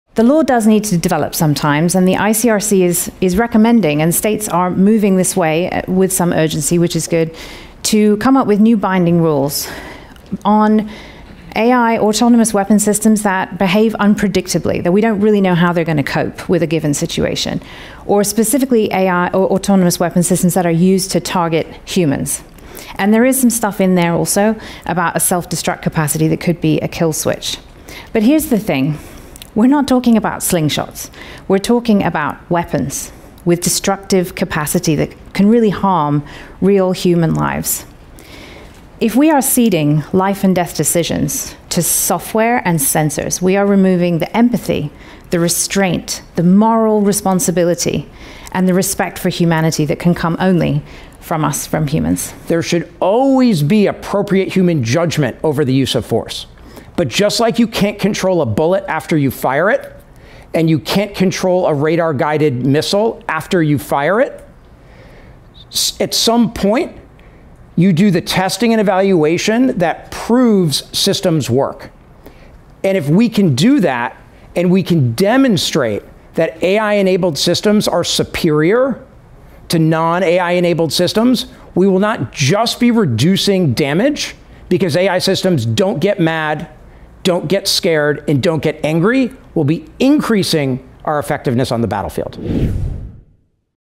Are We Losing Humanity by Letting AI Make Life-and-Death Decisions?
If we hand life-and-death decisions to software, do we lose the empathy and moral restraint that only humans can provide? Or could AI weapons actually reduce harm by acting without fear, anger, or revenge?